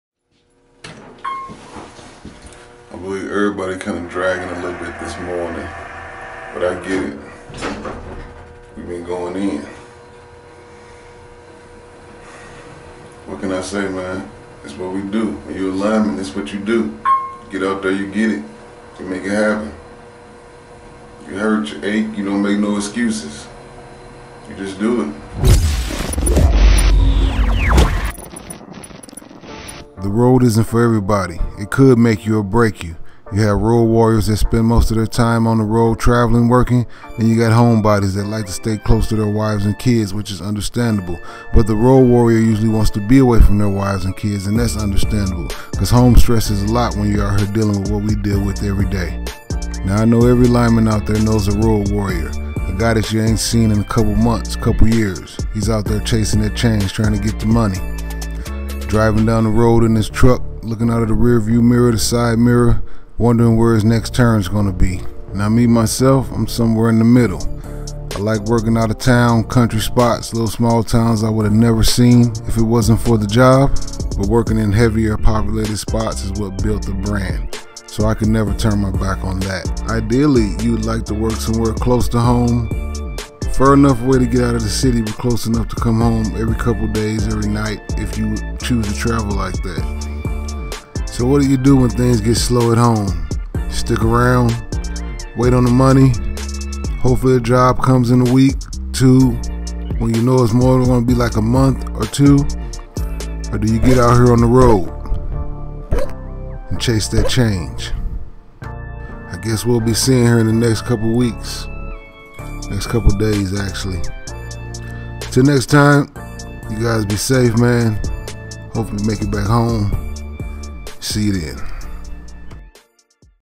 Lineman Vlog - Twill Contracting. 2 Mins With Tee
This Segment of the channel will be something different from the usual. 2 Mins with Tee will be just as it says. We will discuss different things going on  in the business and ways to adjust to survive.The first video is about whats going on here with work in my area, should we wait it out or chase the money and return home in the second quarter of 2020?